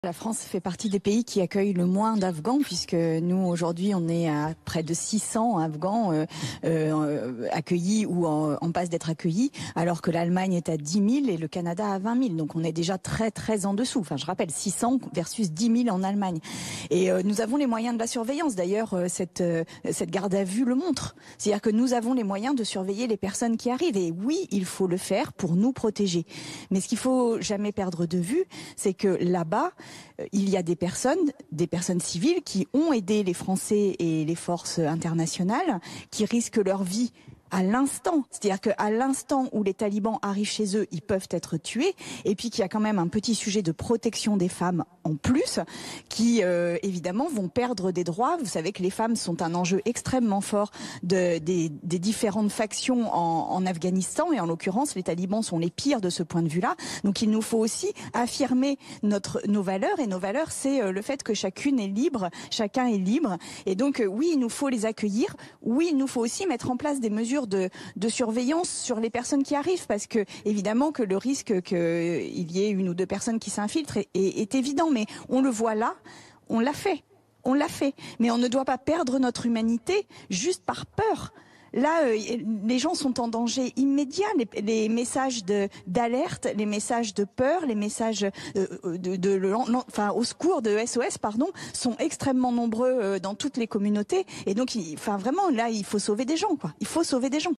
Pour l'accueil des personnes réfugiées en France - Sandrine Rousseau sur BFMTV
Sandrine Rousseau est candidate à la primaire écologiste pour les présidentielles 2022.
Économiste de l’environnement et vice-présidente de l’Université de Lille, elle fut numéro 2 et porte-parole d’EELV, ainsi que vice-présidente du Conseil régional du Nord-Pas-de-Calais.
Militante écoféministe, elle a fondé l’association d’entraide entre victimes de violences sexuelles “En Parler”.

Primaire écologiste, radicalité, écoféminisme, dominations, crise climatique, Afghanistan, accueil, réfugiés, migrants, France, migrations, humanisme